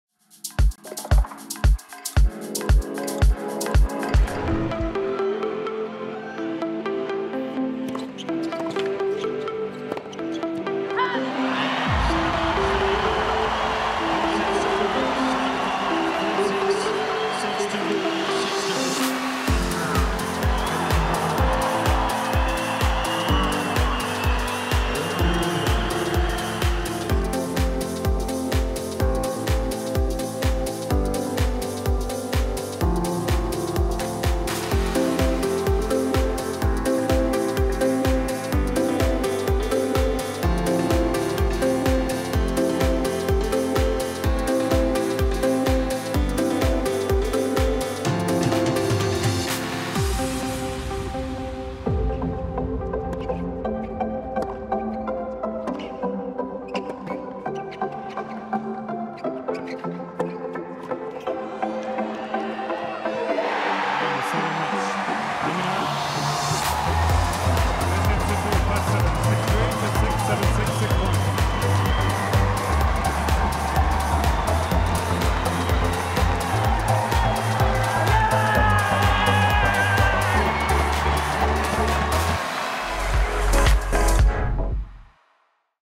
Top 3 Priceless moments | Australian Open 2017
Counting down our top 3 moments for the first day of the Australian Open 2017.